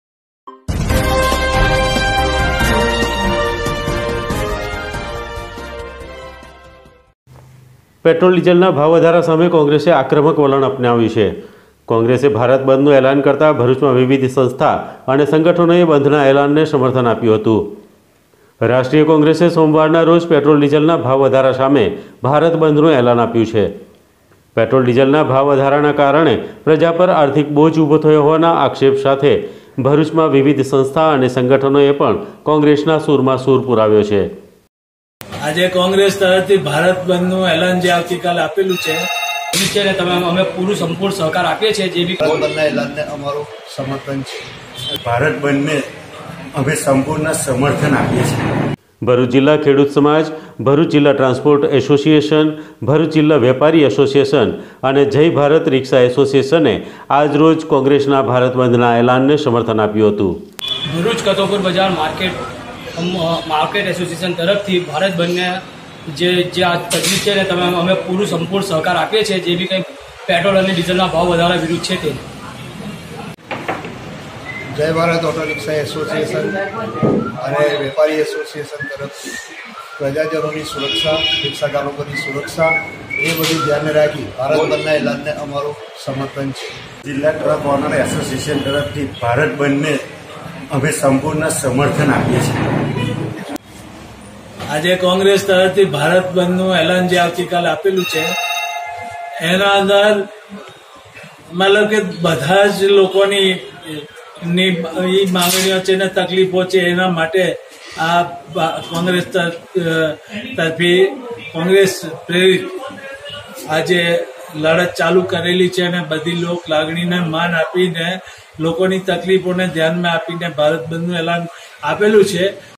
BHARUCHNEWS - આવતી કાલે 10 સપ્ટે. કોંગ્રેસ દ્વારા ભારત બંધને ભરૂચના વિવિધ સંગઠનોએ ટેકો જાહેર કર્યો
BHARUCH - Tomorrow on 10th september 2019 bharat bandh call by congress against fuel and gases price increase is been supported by various associations in bharuch district.